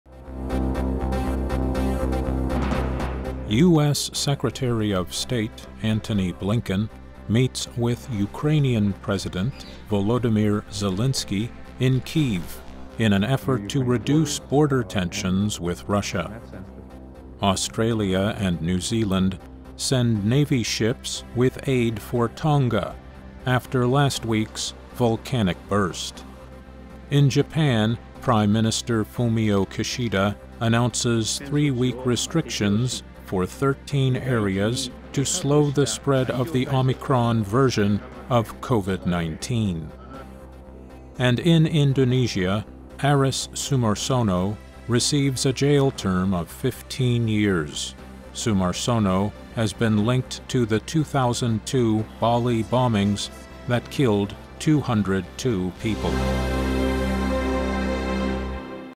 VOA60 January 19 2022
U.S. Secretary of State Antony Blinken meets with Ukrainian President Volodymyr Zelenskiy in Kyiv in an effort to reduce border tensions with Russia.

Australia and New Zealand send navy ships with aid for Tonga after last week's volcanic burst.

In Japan, Prime Minister Fumio Kishida announces three-week restrictions for 13 areas to slow the spread of the Omicron version of COVID-19.

And in Indonesia, Aris Sumarsono receives a jail term of 15 years. Sumarsono has been linked to the 2002 Bali bombings that killed 202 people.